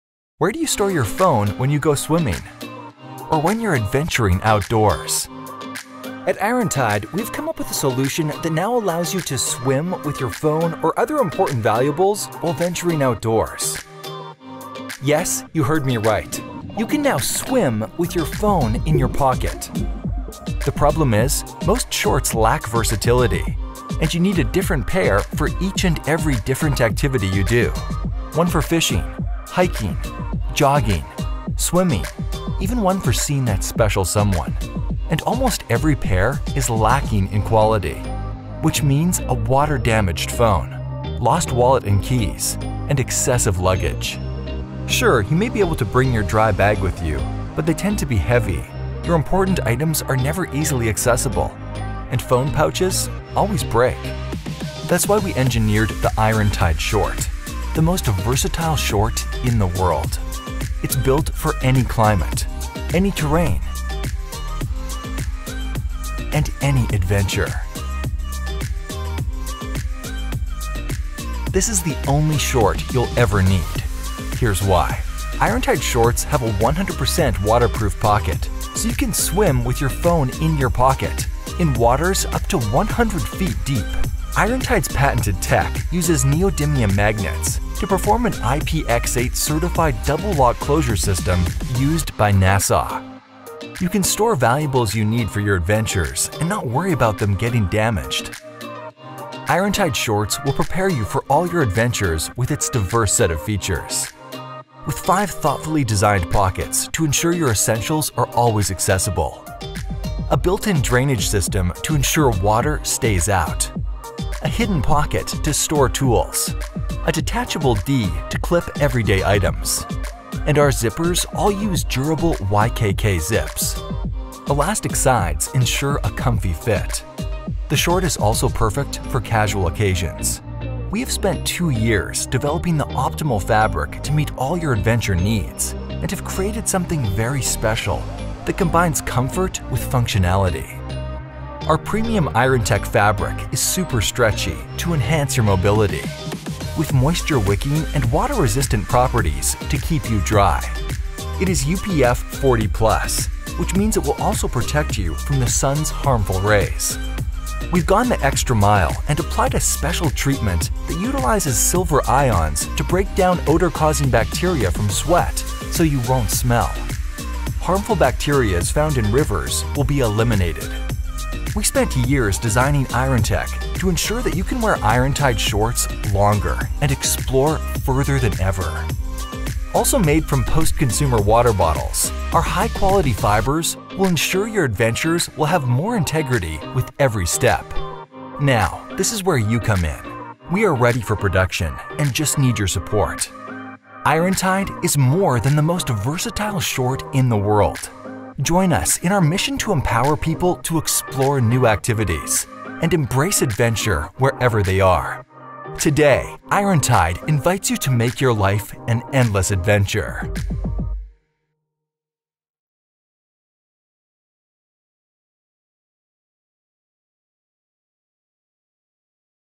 Best of Kickstarter 💡 Irontide Adventure Shorts
Designed for adventurers. Keep your valuables safe, secure, and dry while you explore the outdoors. IPX-8 certified. Submersible to 100 feet.